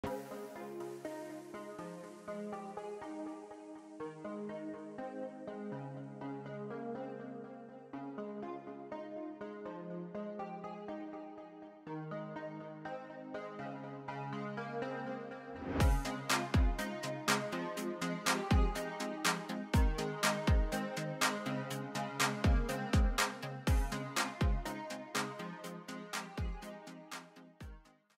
Learn ASL: Kid, Children, Child
Wax on, Wax off! An ASL lesson inspired by today’s home reno project.